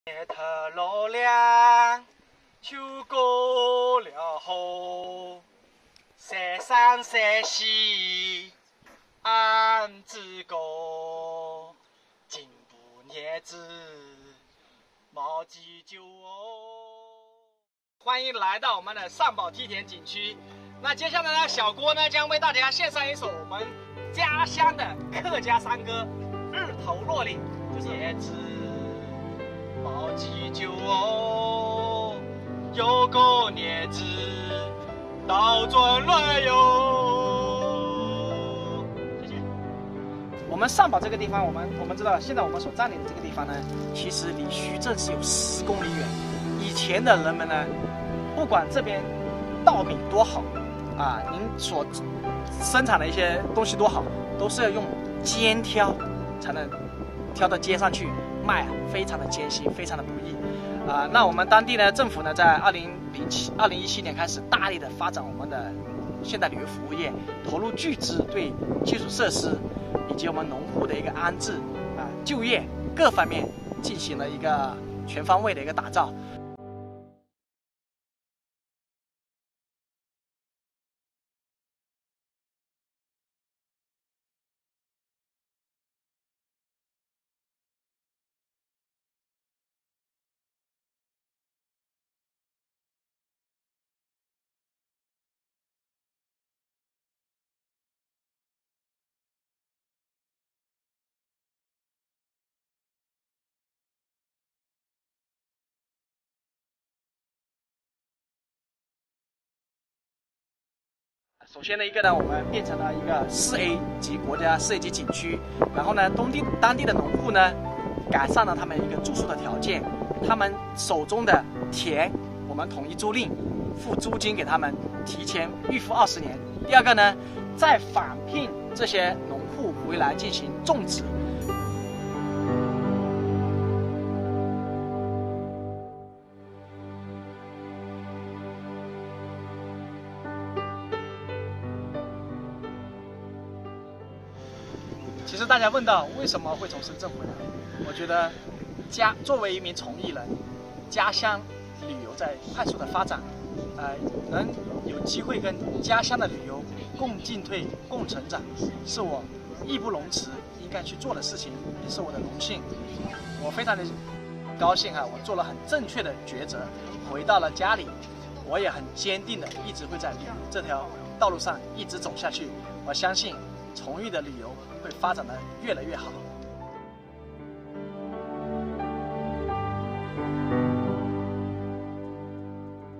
深圳金牌导游回崇义助力“最大客家梯田”运营：做了正确选择
近日，江西赣州。崇义人郭煜曾经在深圳从事旅游业多年，3年前回到家乡，助力“世界最大客家梯田”上堡梯田景区运营建设。他表示，家乡旅游快速发展，自己做了正确的选择。（素材来源：澎湃新闻）